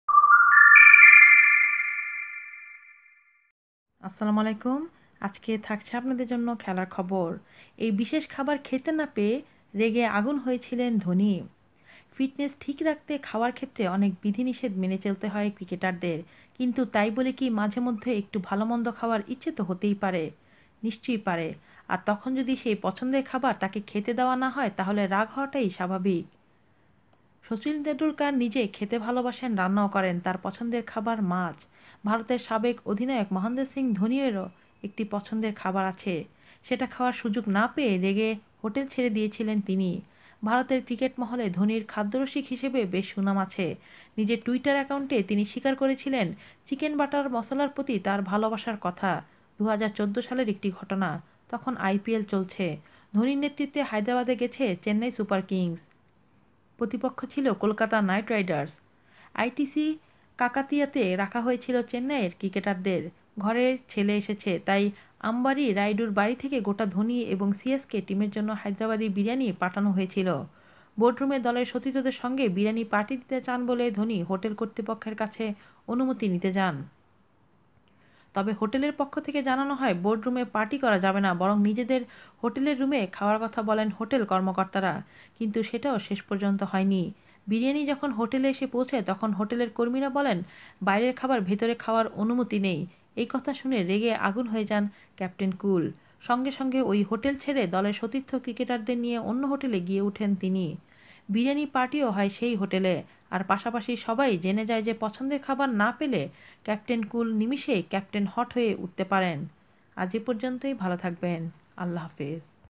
পছন্দের খাবার না পেলে 'ক্যাপ্টেন কুল' নিমিষেই 'ক্যাপ্টেন হট' কে সে ???
ফিটনেস ঠিক রাখতে খাওয়ার ক্ষেত্রে অনেক বিধিনিষেধ মেনে চলতে হয় ক্রিকেটারদের। কিন্তু তাই বলে কি মাঝে মধ্যে একটু ভালো-মন্দ খাওয়ার ইচ্ছে হতে পারে না? নিশ্চয়ই পারে। আর তখন যদি সেই পছন্দের খাবার তাকে খেতে দেওয়া না হয়; তাহলে রাগ হওয়াটাই স্বাভাবিক।

ক্রিকেট ঈশ্বর শচীন টেন্ডুলকার নিজে খেতে ভালোবাসেন, রান্নাও করেন।  তার পছন্দের খাবার মাছ। ভারতের সাবেক অধিনায়ক মহেন্দ্র সিং ধোনিরও একটি পছন্দের খাবার আছে; সেটা খাওয়ার সুযোগ না পেয়ে রেগে হোটেল ছেড়ে দিয়েছিলেন তিনি!

ভারতের ক্রিকেট মহলে ধোনির খাদ্যরসিক হিসেবে বেশ সুনাম আছে। নিজের টুইটার অ্যাকাউন্টে তিনি স্বীকার করেছিলেন চিকেন বাটার মশলার প্রতি তার ভালোবাসার কথা। ২০১৪ সালের একটি ঘটনা। তখন আইপিএল চলছে। ধোনির নেতৃত্বে হায়দরাবাদে গেছে চেন্নাই সুপার কিংস।  প্রতিপক্ষ ছিল কলকাতা নাইট রাইডার্স। আইটিসি কাকাটিয়াতে রাখা হয়েছিল চেন্নাইয়ের ক্রিকেটারদের।